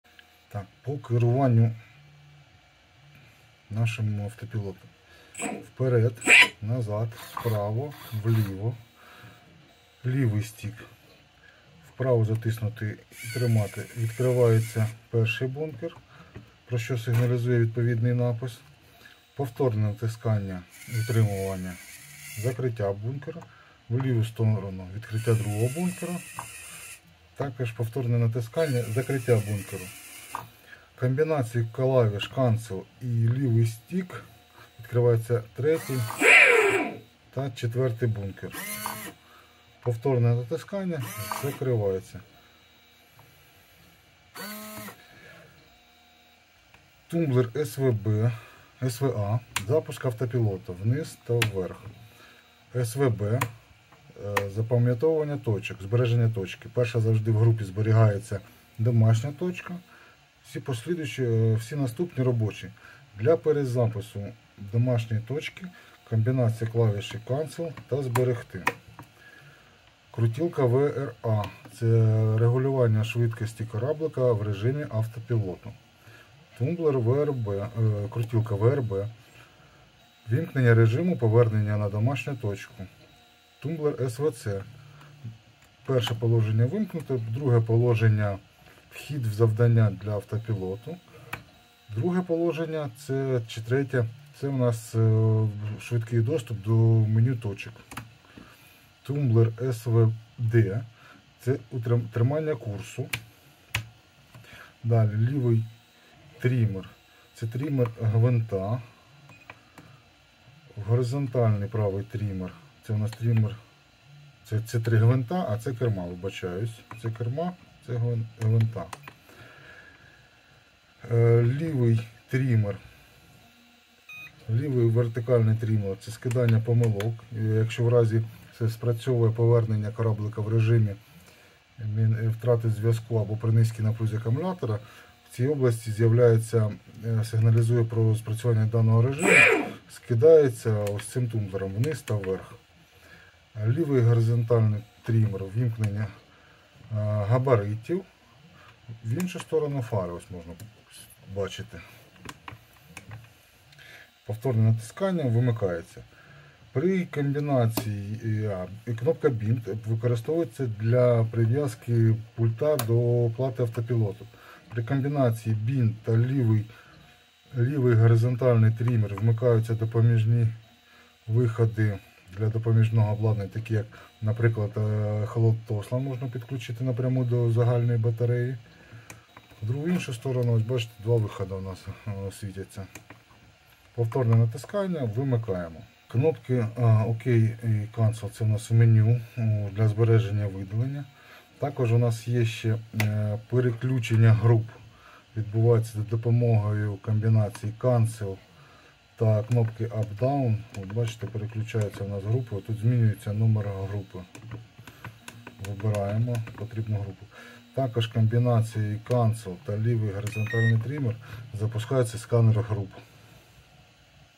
Автопілот Гідра - коротко про керування, автопілот hydra
Інструкція по управлінню автопілотом Гідра. Коротко про та з демонстрацією. Підтримка 4 бункерів.